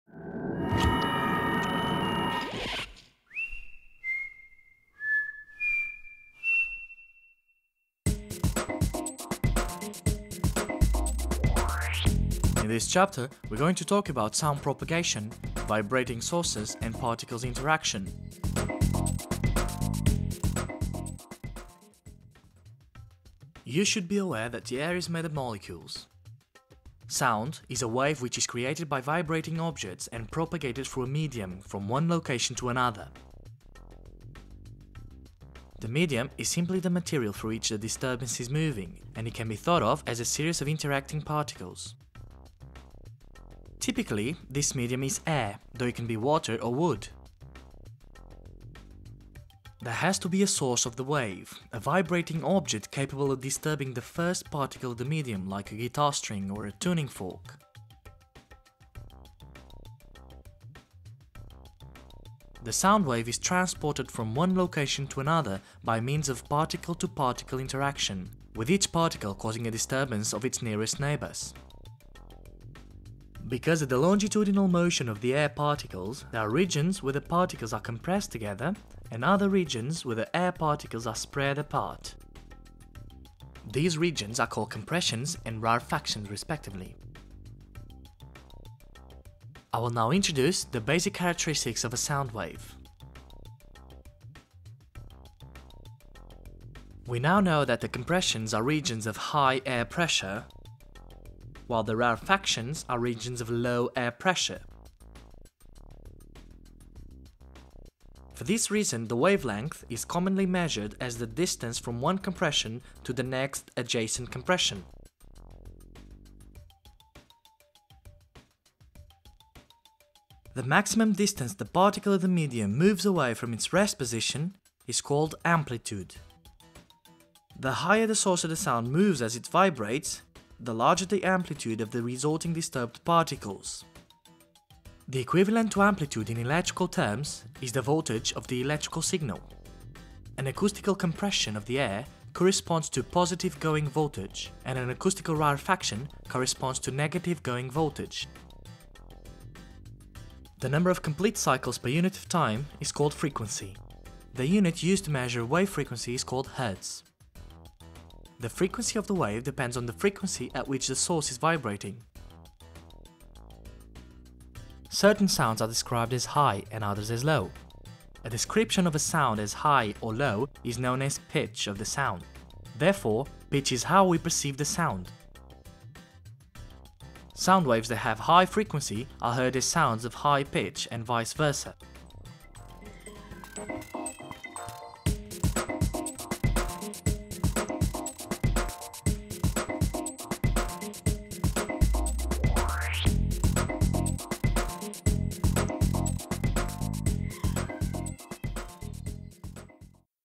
Episode 1 - Sound behaviour part 1
Introduction to sound, propagation, wave characteristics, compressions, rarefactions, wavelength, amplitude, frequency, pitch, tone